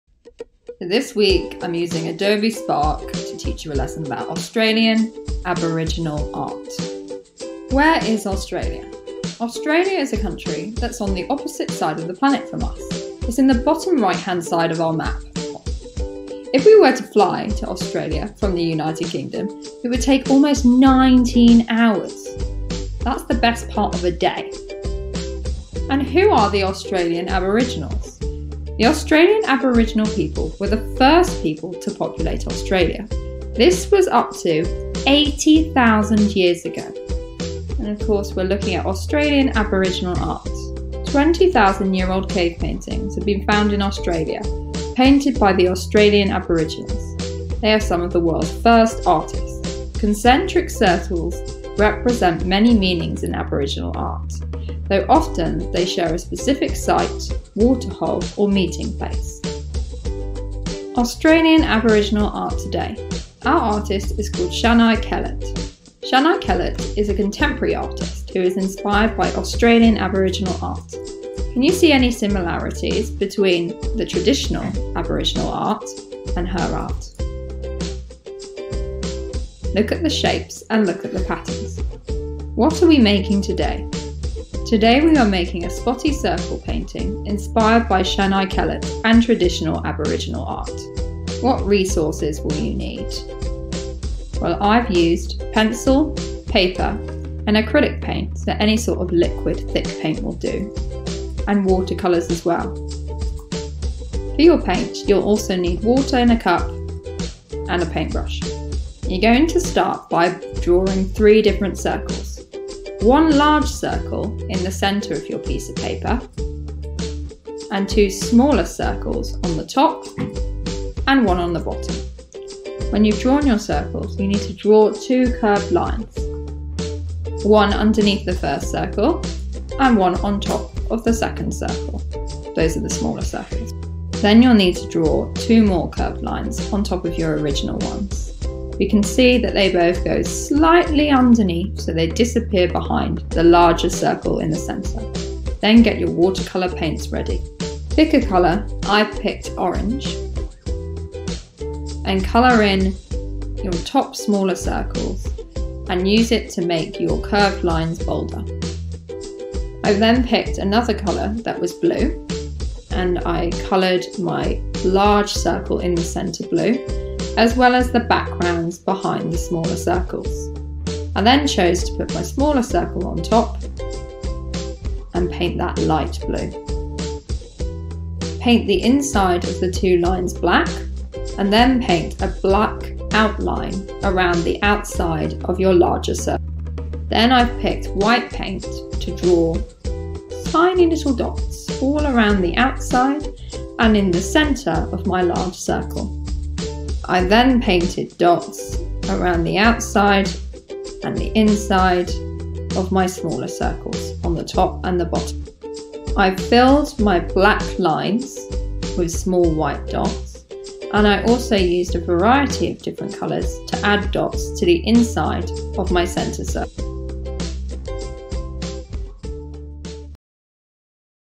Australian Aboriginal Art
Here is a lesson on Australian Aboriginal art, inspired by work by the artist Shanai Kellet.

Remember that you can pause and play the instructions at any time so you have time to catch up with the activity!